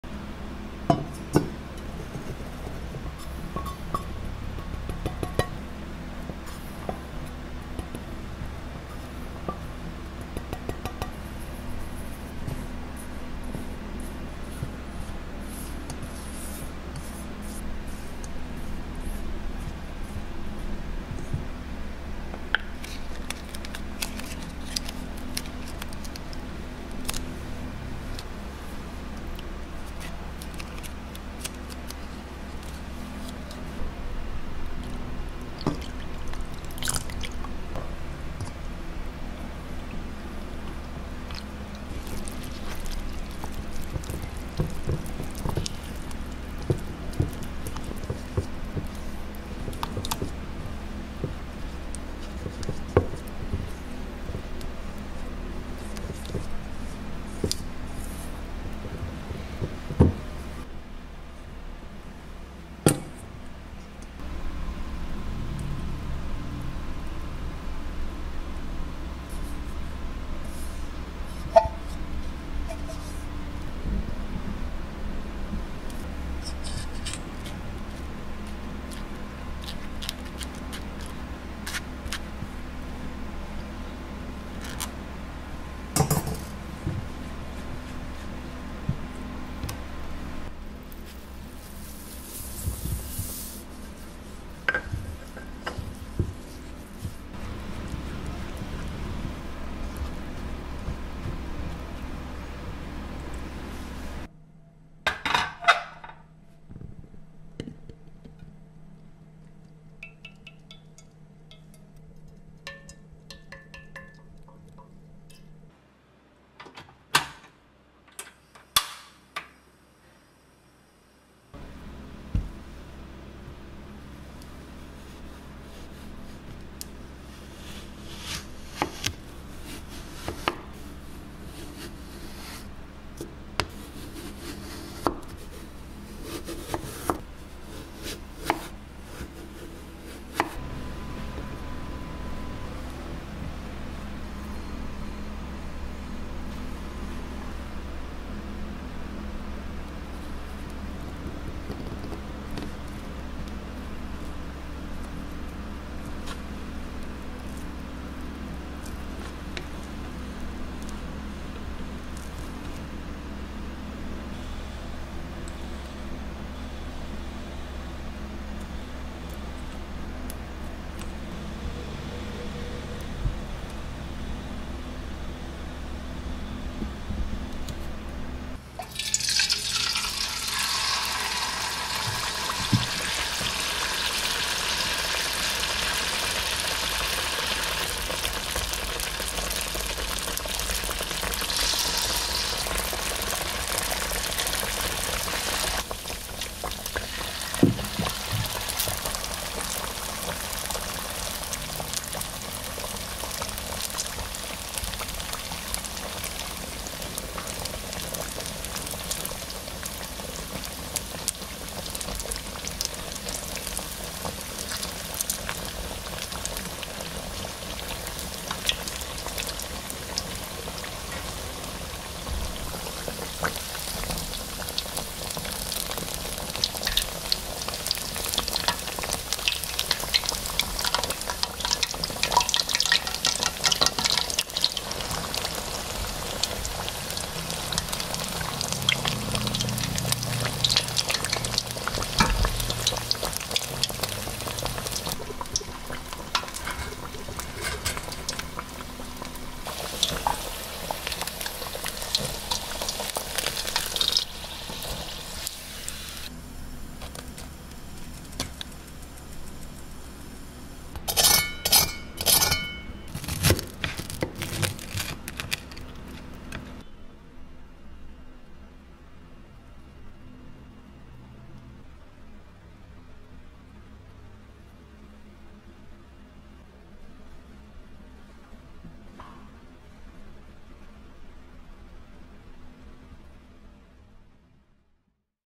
Kue Keranjang / Dodol Cina GORENG | ASMR COOKING
Cara Mengolah Kue Keranjang / Dodol Cina supaya nggak terlalu manis...
DIGORENG AJAAA...